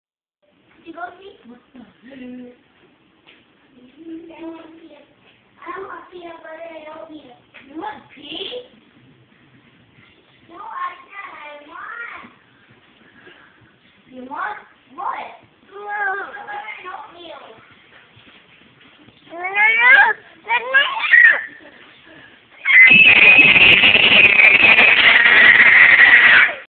video-2011-03-19-15-10-15
This is how my little one gets upset when he can't do something.